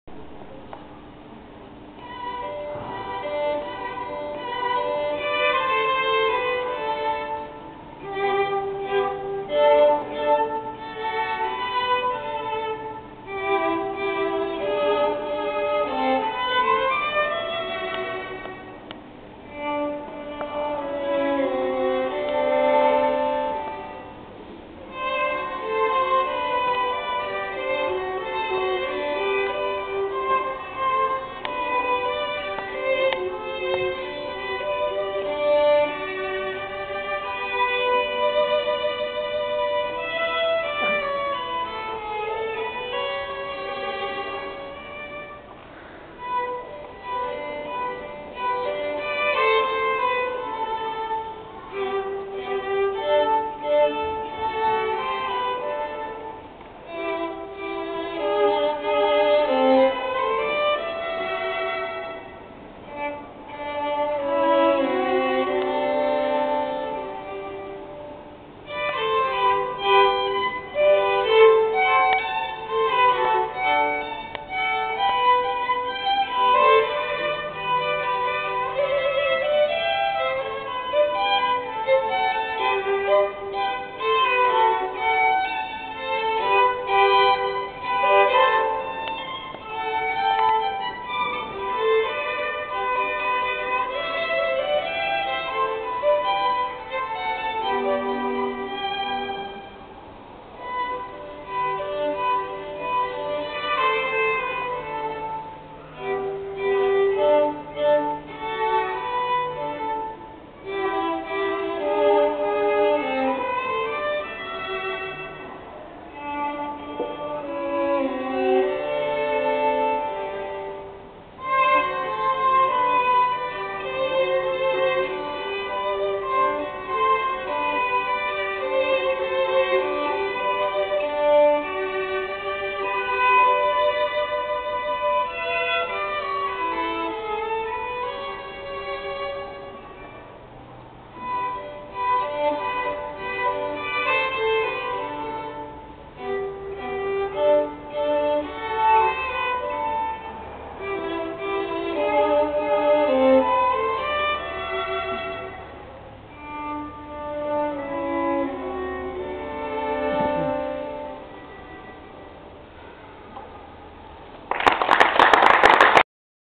Mozart-Zelter Duo-Musequality World Busk 2010
Zelter Duo
Angela Amato ( Violin )
Roberto Albin ( Viola )